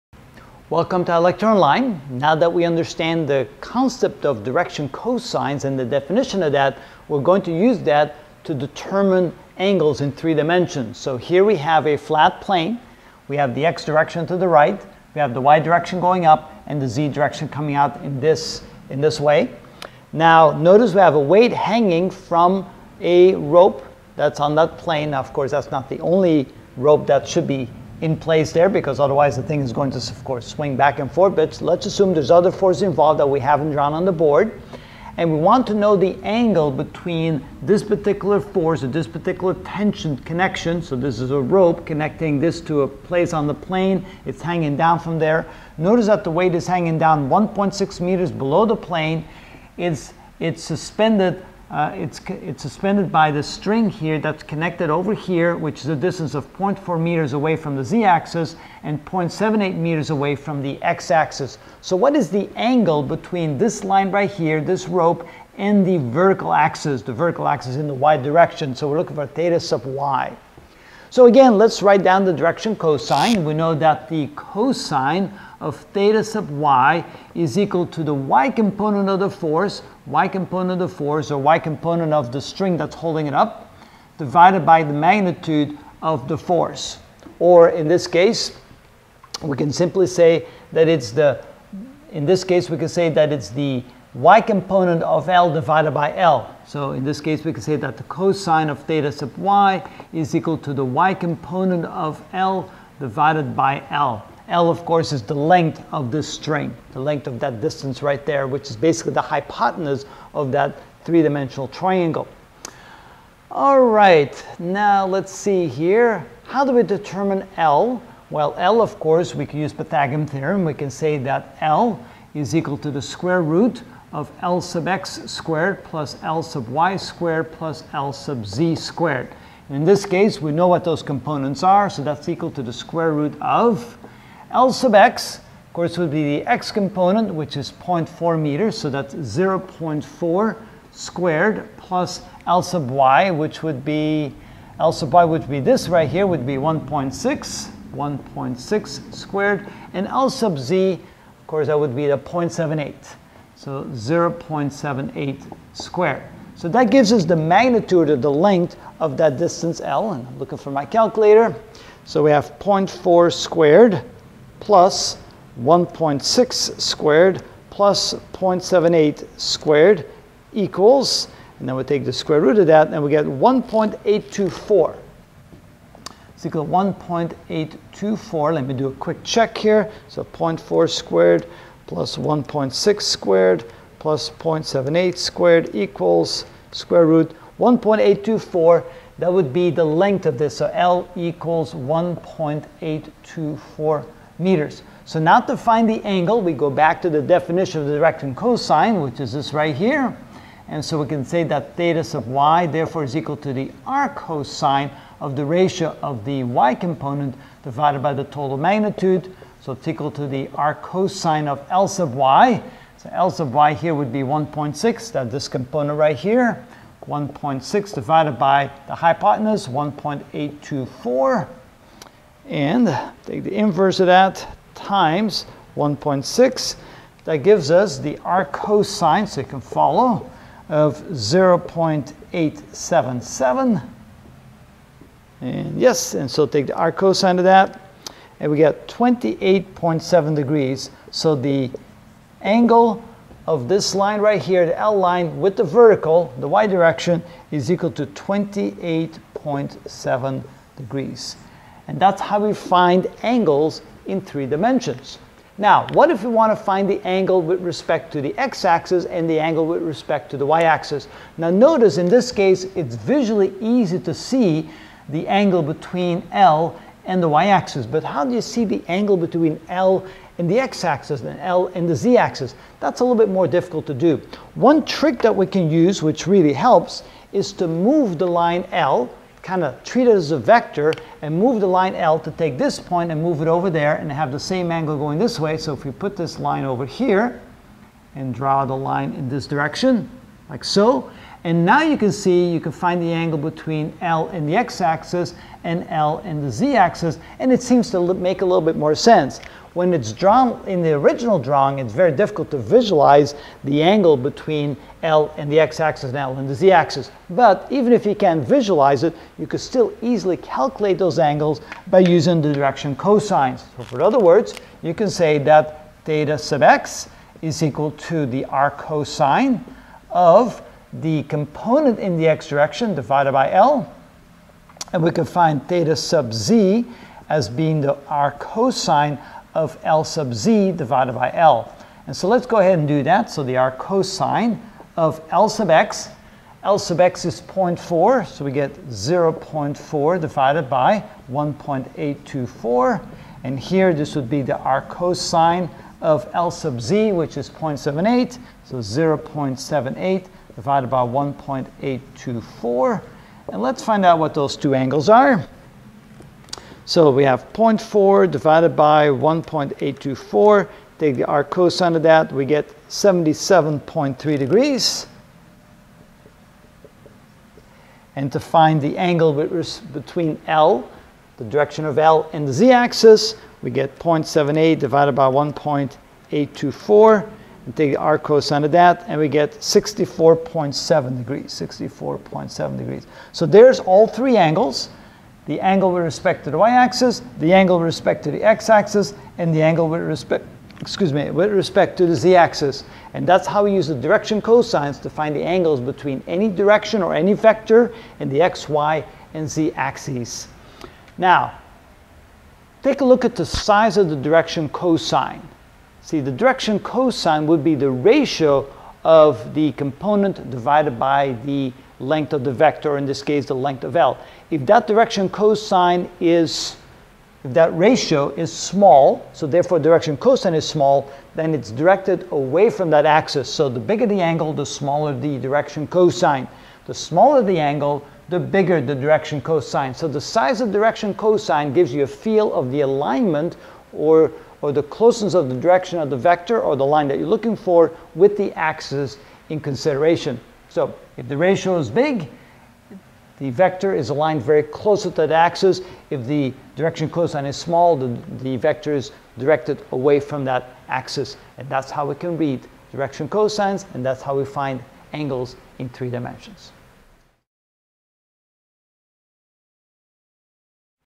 Mechanical Engineering: Particle Equilibrium (16 of 19) Determining Angles in 3-Dimension
In this video I will calculate the 3 angles of a hanging mass in 3-dimensions.